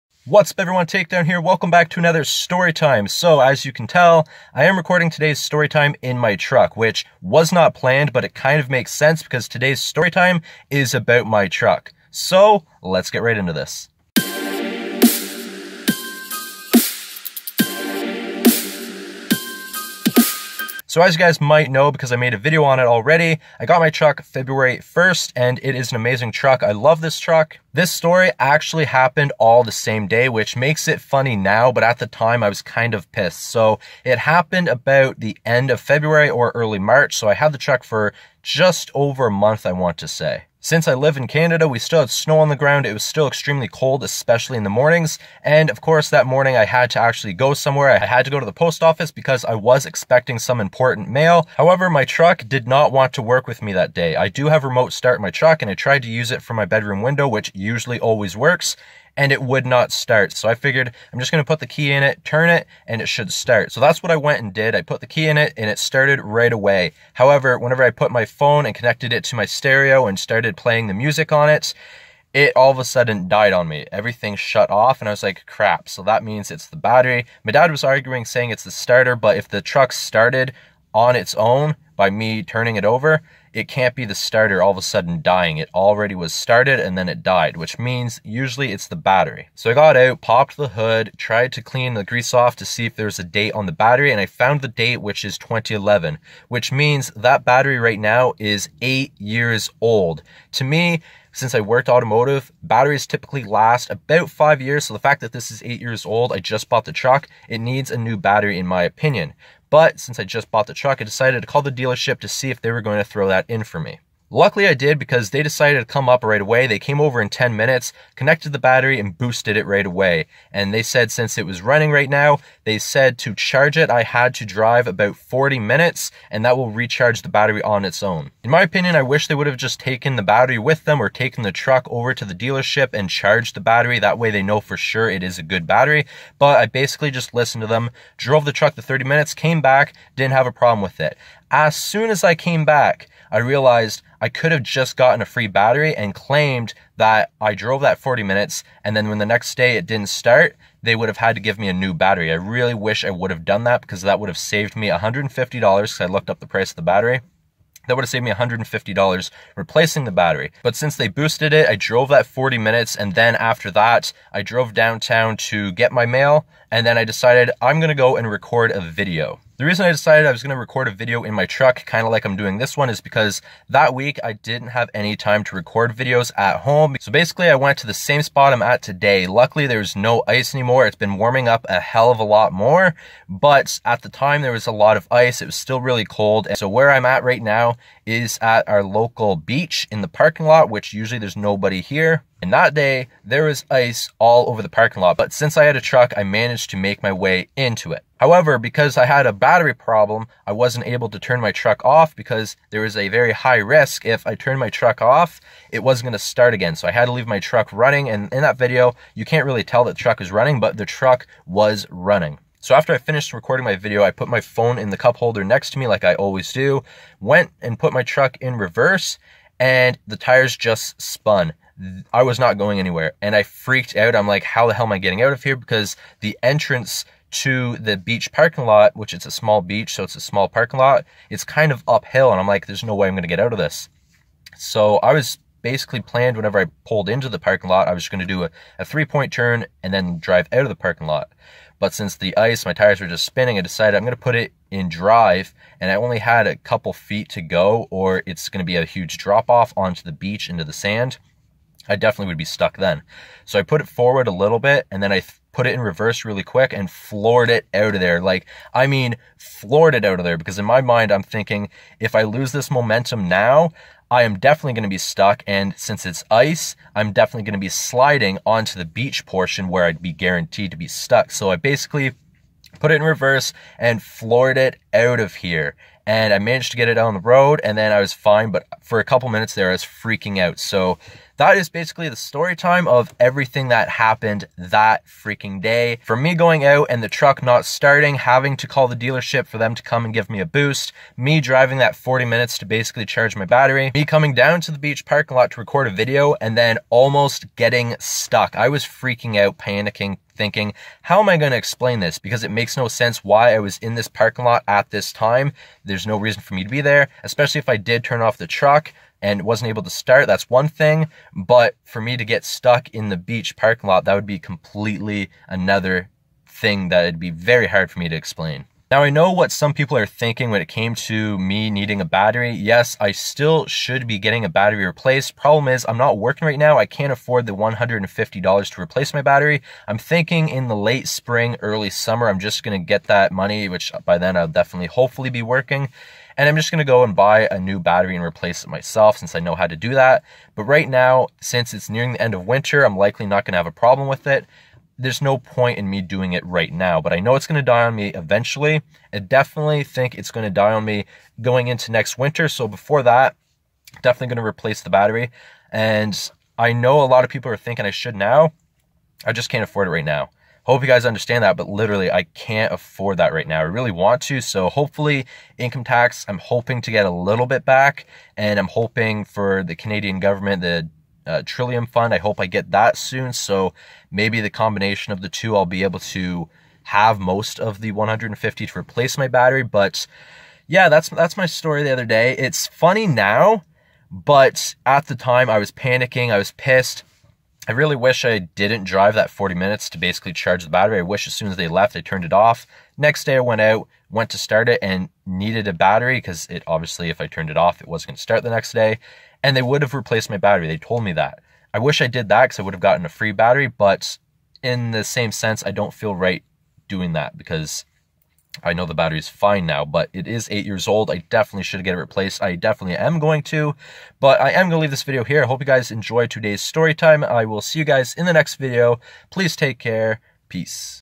STORY TIME || BATTERY PROBLEM & ALMOST GOT IT STUCK
Back with another story time but today its about my truck. I bought my 2010 GMC Sierra in the beginning of February and this happened after one month of me owning it